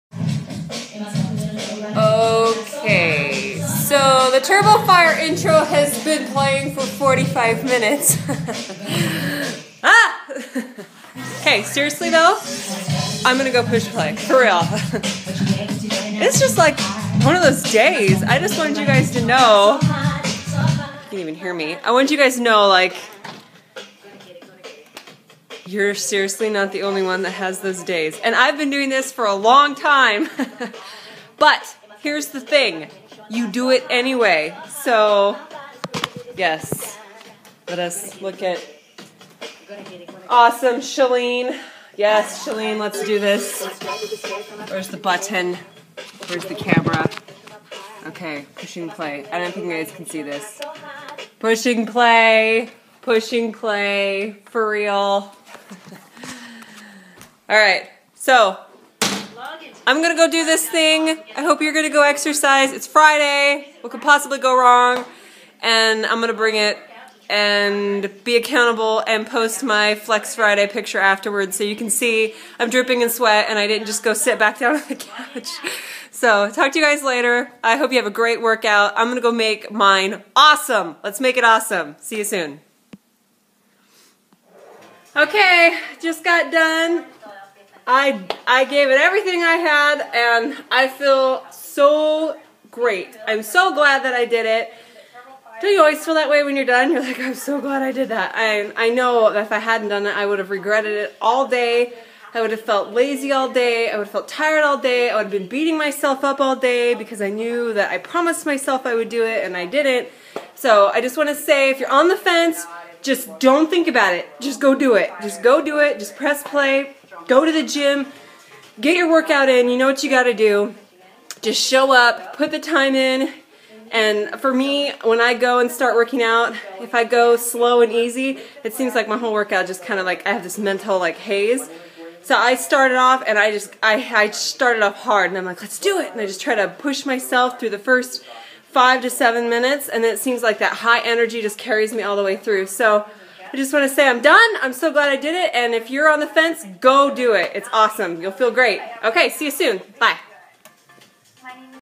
I Don't Wanna Workout! WATCH THIS!
Ever feel like, "ugh, I just don't wanna workout!"  Me too!  Learn what to do when you just don't feel like exercising.  My 3 tricks for getting your best workout ever!